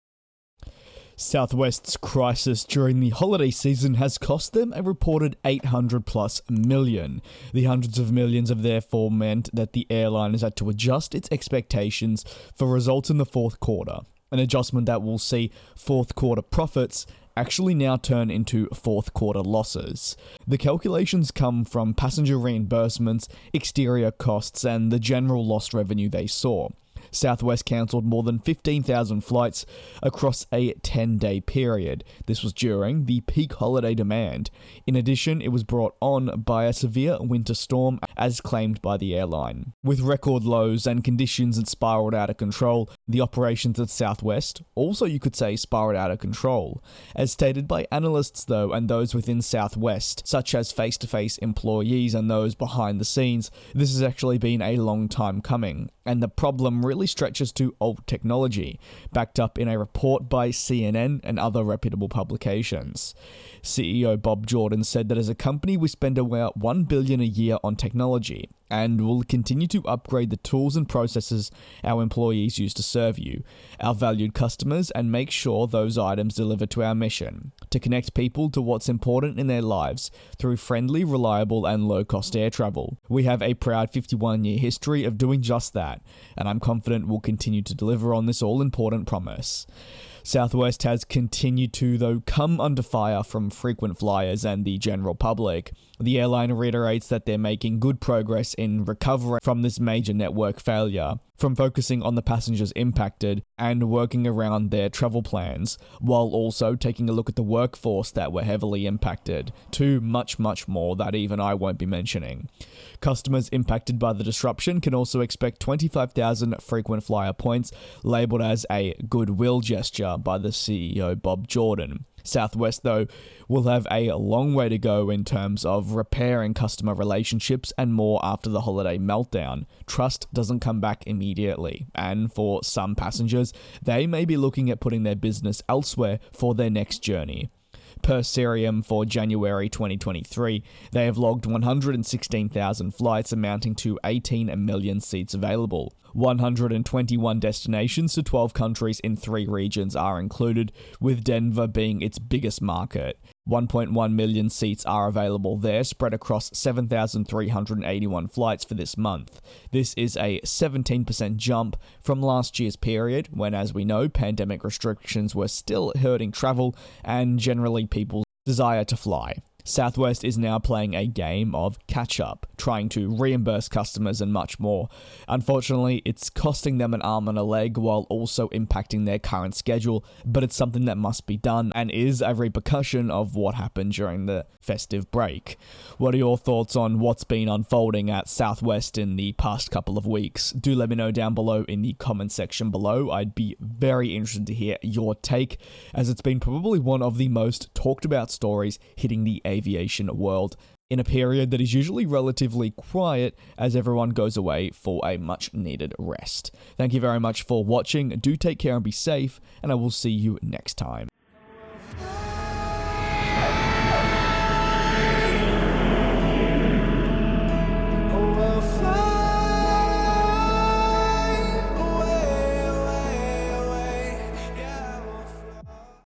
Bad News For Southwest
Bad news once again at Southwest as the airline announces that the December meltdown that led to 15,000+ flights being cancelled and their entire network collapsing will cost them hundreds of millions and impact their fourth-quarter results. This is the latest updated on the airline and incident that made headlines globally for all the wrong reasons.